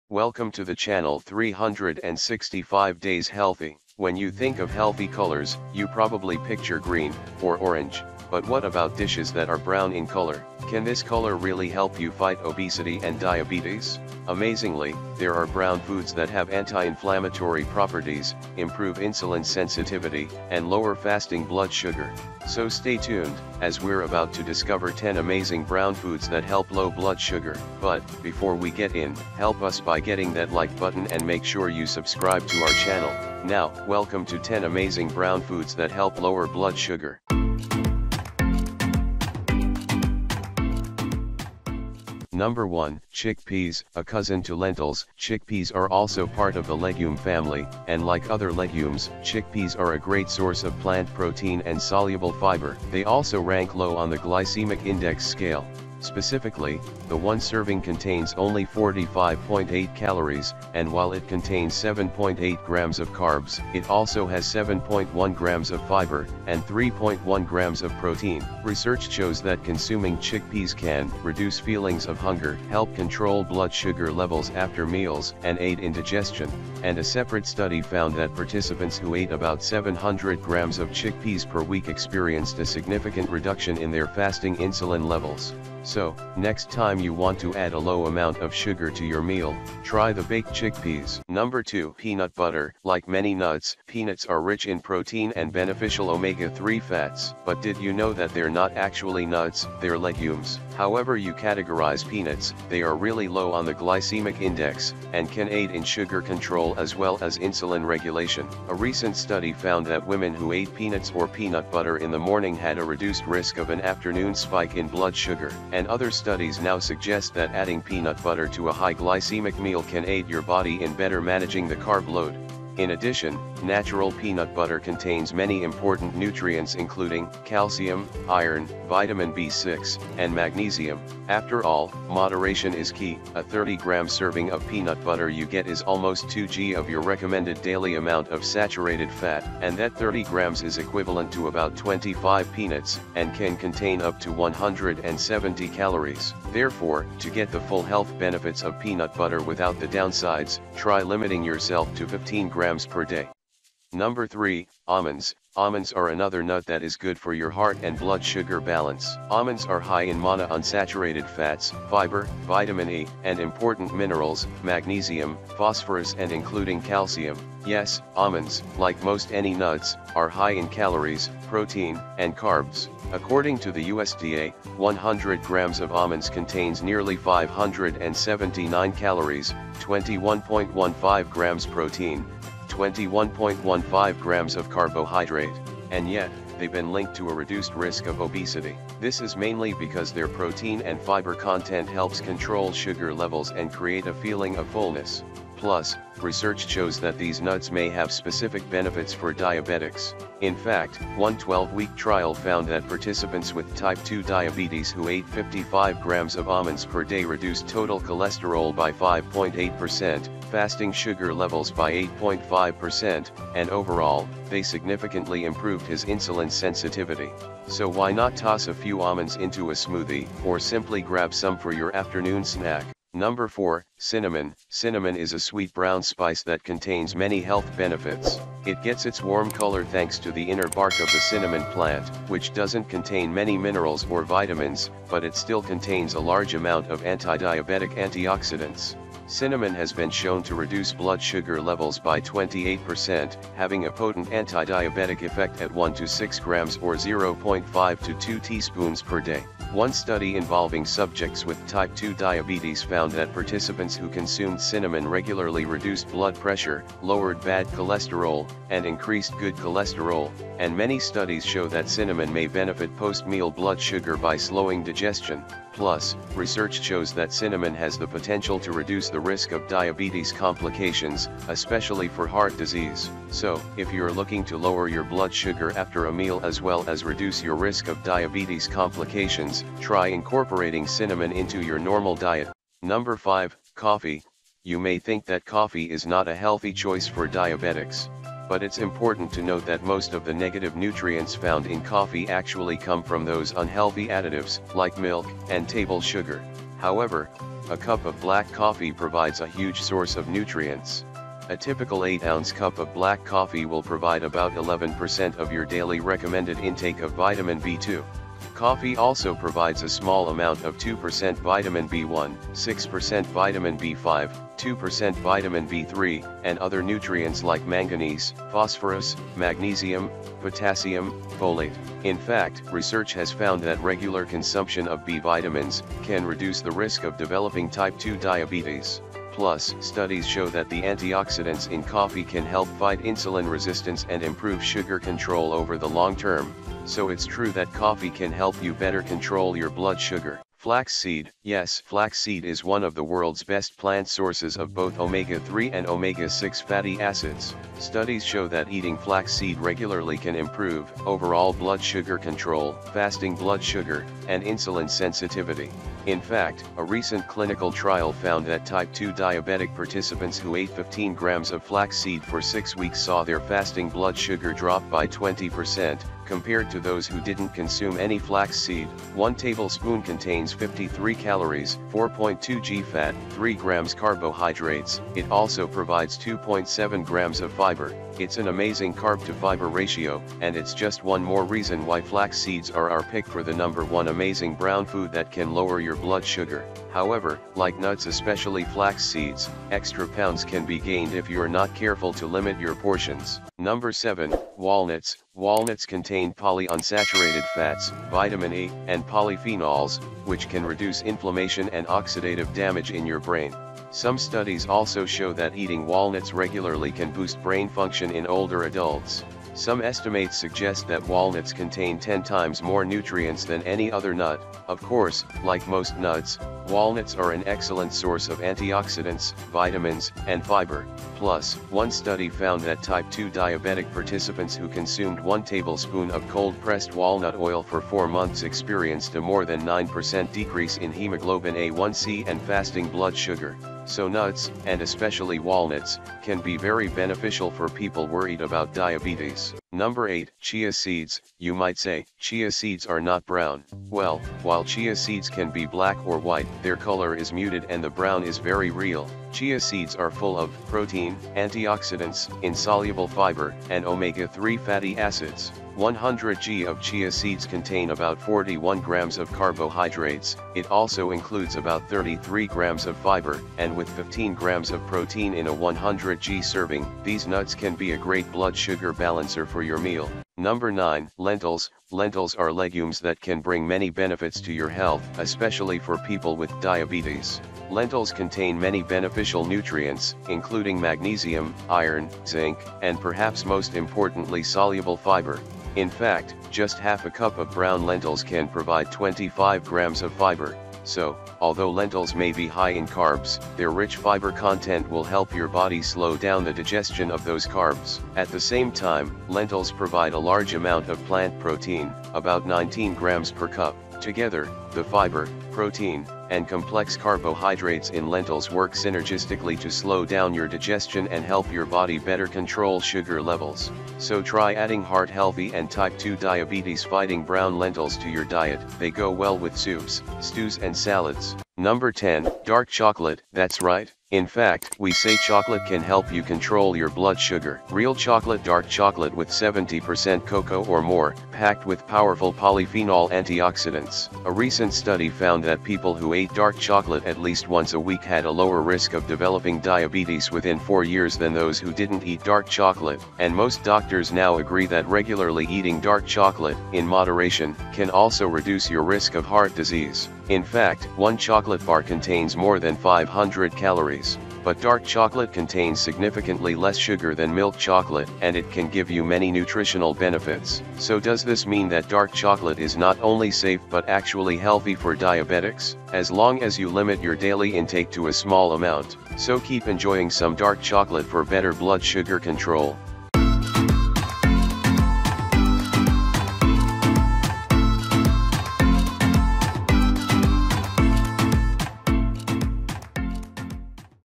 10 Brown Foods That Fight Diabetes | Foods That Lower Blood Sugar | Best Foods For Diabetics
Discover 10 Brown Foods That Lower Blood Sugar-365 Days Healthy
When you think of healthy colors, you probably picture green, or orange. But what about dishes that are brown in color? Can this color really help you fight obesity and diabetes?
Amazingly, there are brown foods that have anti-inflammatory properties, improve insulin sensitivity, and lower fasting blood sugar.

✅ 𝗬𝗼𝘂 𝗠𝗮𝘆 𝗕𝗲 𝗜𝗻𝘁𝗲𝗿𝗲𝘀𝘁𝗲𝗱 ✅
𝘙𝘦𝘷𝘦𝘳𝘴𝘦 𝘊𝘩𝘳𝘰𝘯𝘪𝘤 𝘒𝘪𝘥𝘯𝘦𝘺:

Content:
00:00 Welcome
00:42 Chickpeas For Diabetics
01:32 Peanut Butter For Diabetics
02:34 Almonds For Diabetics
03:43 Cinnamon For Diabetes
04:44 Coffee For Diabetics
05:40 Flax Seeds For Diabetics
06:34 Walnuts For Diabetics
07:20 Chia Seeds For Diabetics
07:51 Lentils For Diabetics
08:41 Dark Chocolate For Diabetics